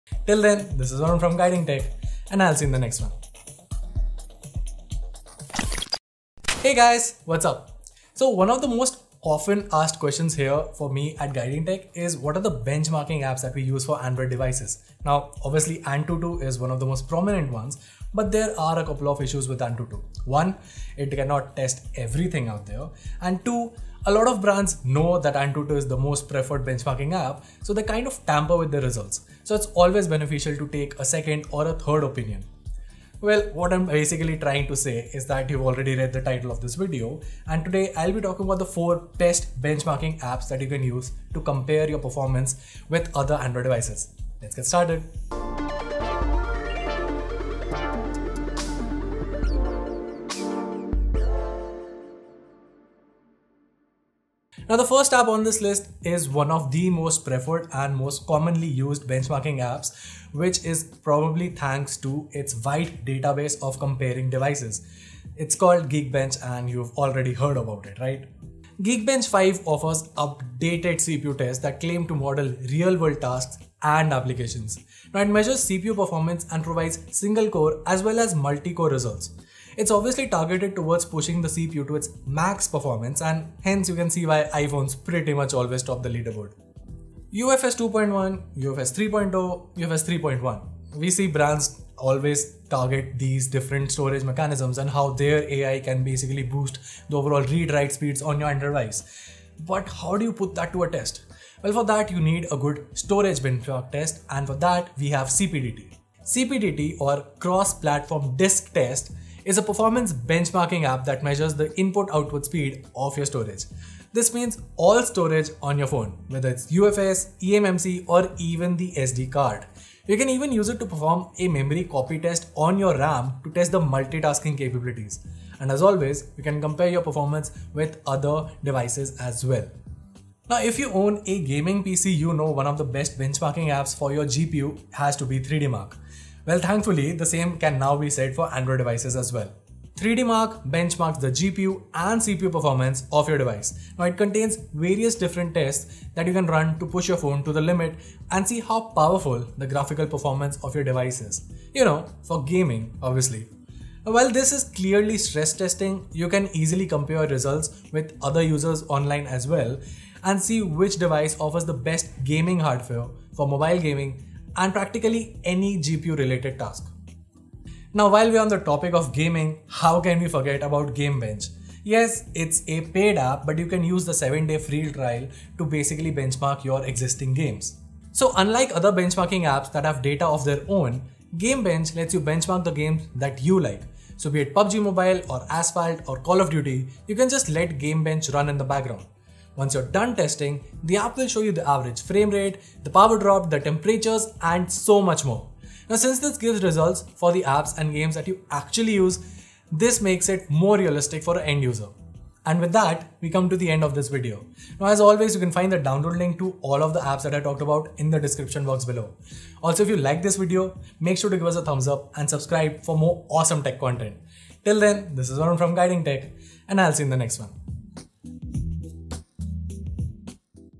Top 4 Best Benchmarking Apps for Android | AnTuTu Alternatives in 2020 | Guiding Tech
Are you looking to push your device to the limits and see how it fares against other Android phones? Today, we bring to you the Top 4 best benchmarking apps for Android in 2020 that you can check out!